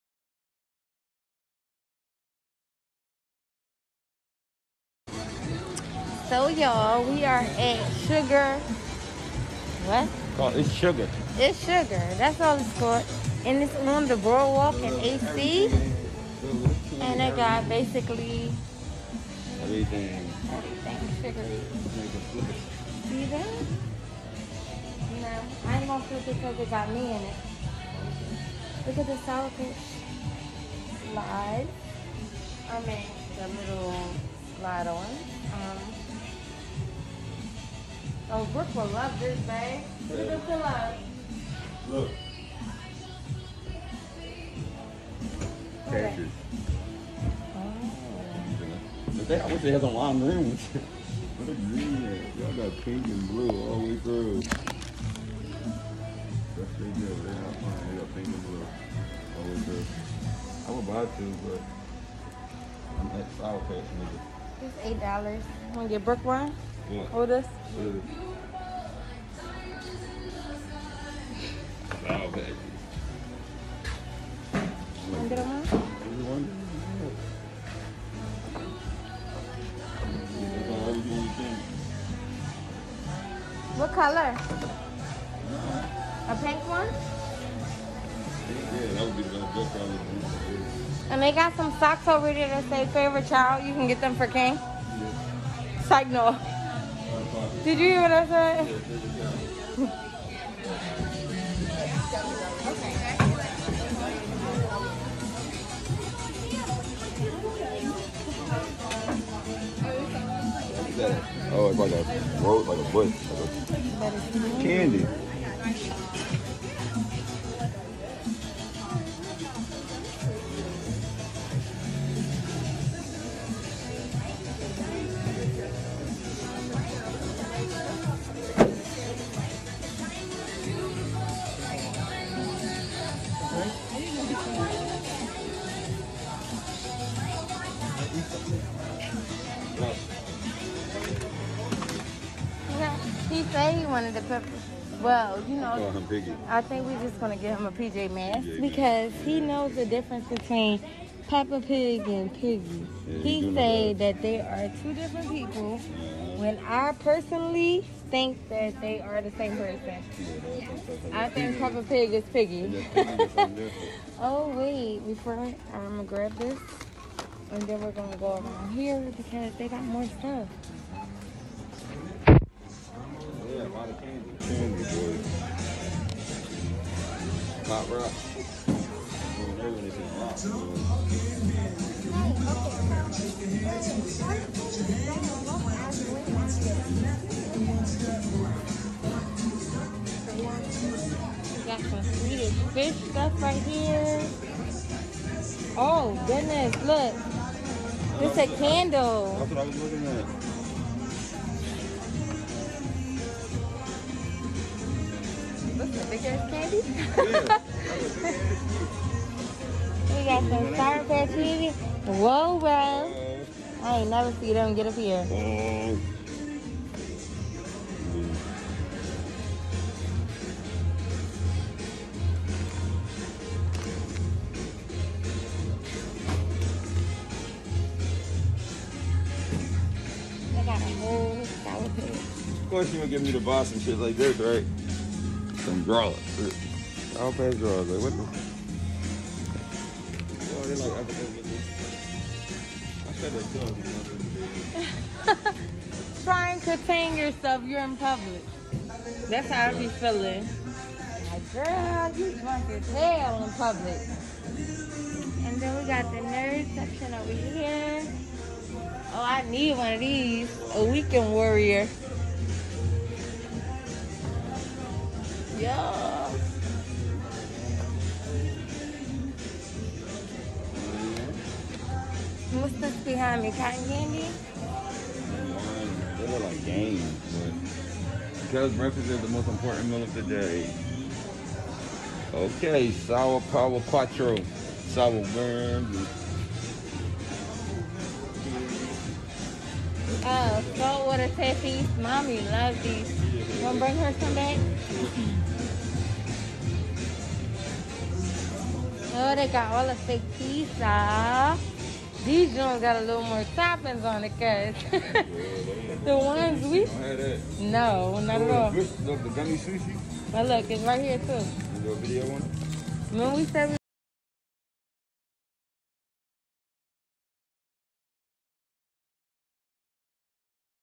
SUGAR🍭🍬🍫 Atlantic City Candy store
Sugar is a candy store located in Atlantic City New Jersey on the Boardwalk
Every candy you could ever think of they have